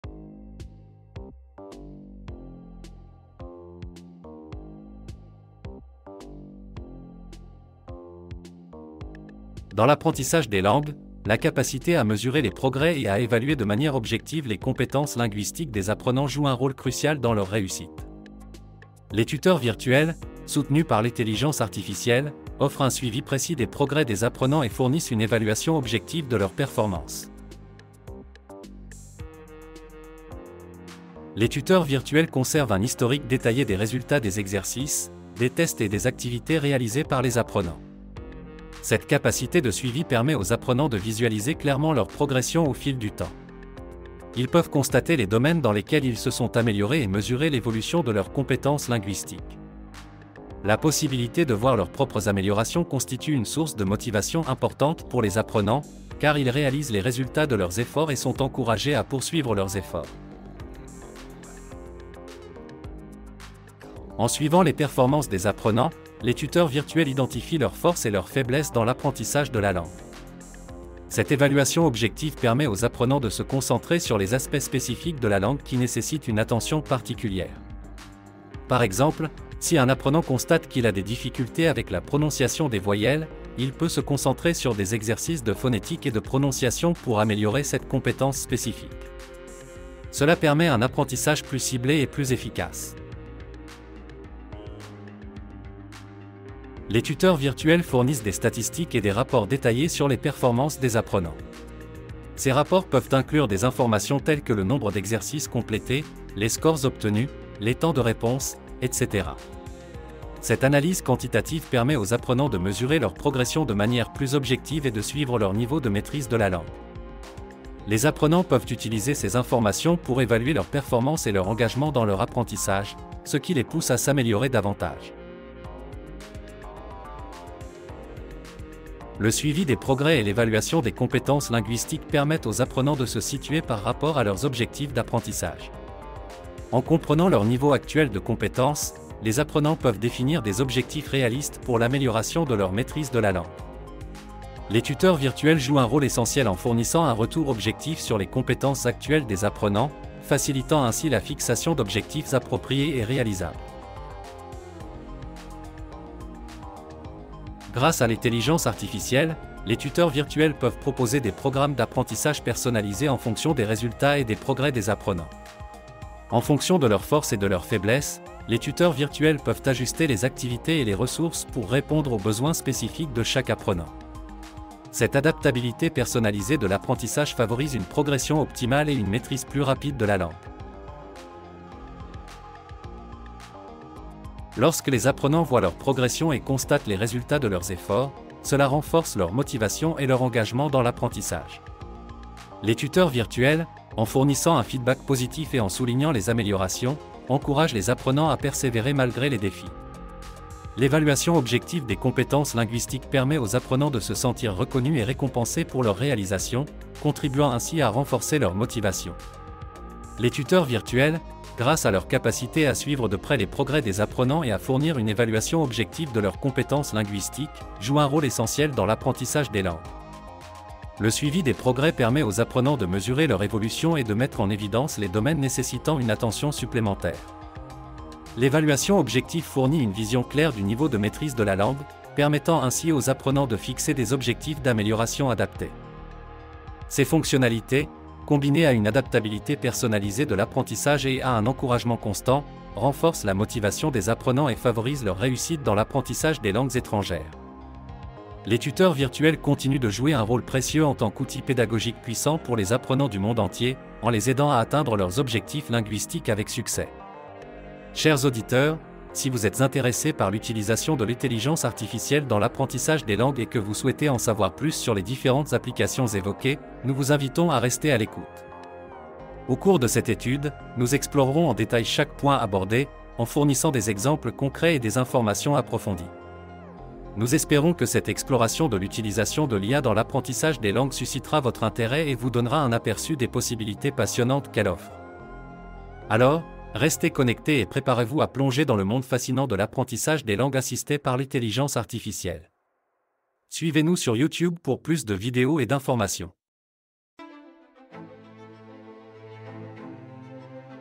09 Tuteurs virtuels - Suivi des progrès et évaluation
🎙️🌐 Nouvel épisode du podcast : "Utilisation de l’intelligence artificielle dans l’apprentissage des langues" 🌐🎙️

Dans cet épisode, nous abordons un sujet essentiel pour les apprenants de langues : "Suivi des progrès et évaluation". Les tuteurs virtuels, soutenus par l'intelligence artificielle, jouent un rôle crucial dans la réussite de l'apprentissage des langues en offrant un suivi précis des progrès et une évaluation objective des compétences linguistiques.

📈🏆 Le suivi des progrès permet aux apprenants de visualiser leur évolution au fil du temps et de se motiver grâce à leurs améliorations. Les tuteurs virtuels identifient également les forces et les faiblesses, ce qui permet un apprentissage ciblé et efficace.

📊💡 Grâce aux rapports détaillés, les apprenants peuvent analyser leurs performances de manière quantitative et évaluer leur niveau de maîtrise de la langue. Cela les aide à fixer des objectifs d'apprentissage réalistes et réalisables.

💪💻 L'adaptation personnalisée de l'apprentissage est une autre force des tuteurs virtuels. Ils proposent des programmes adaptés aux besoins spécifiques de chaque apprenant pour une progression optimale.

🎉💬 En fournissant un feedback positif et en soulignant les améliorations, les tuteurs virtuels encouragent et motivent les apprenants à persévérer malgré les défis.

🌟 Ne manquez pas cet épisode captivant qui vous éclairera sur le rôle essentiel des tuteurs virtuels dans l'apprentissage des langues.